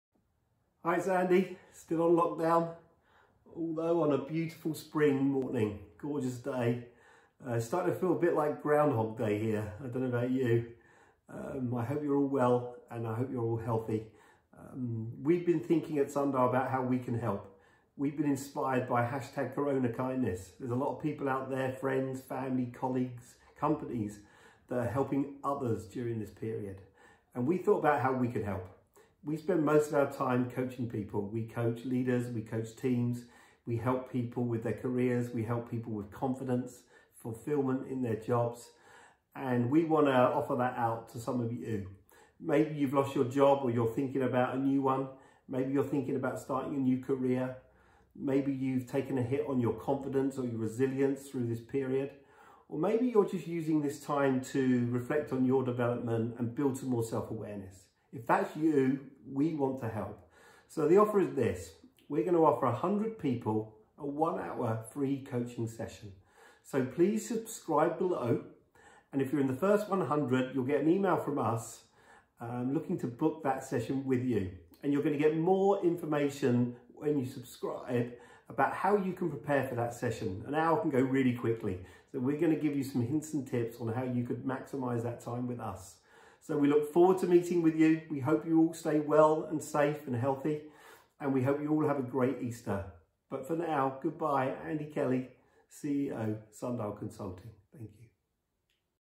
FREE Personal Coaching Hour for First 100 | Please Share with Anyone This Could Help
Maybe you've lost your job, thinking of a career change or just struggling with your confidence.

A lot of people are in crisis right now - and we want to do our bit to help as many as we can. We're offering a no strings attached, FREE online coaching session to the first 100 people who sign up. We're lucky enough to have a huge wealth of knowledge on change, confidence and career shifts - and we hope to help arm you with what you need to go forward with direction and confidence.

Sign up quickly here to be one of the first 100 and we'll be in touch very soon:

Know a friend or family member who could use a free session?

Help spread the word and share on any social media channels if you can - we want to help as many people as possible.

Thanks for your support, stay safe,
Team Sundial